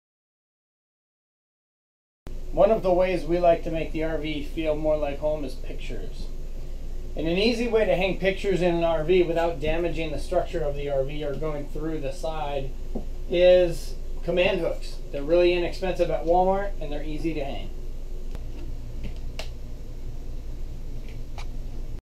RV Command Hooks for Hanging
Do you want to hang things in an RV without doing damage to any of the interior or exterior of the Travel Trailer or Fifth Wheel? Check out this RV Tip.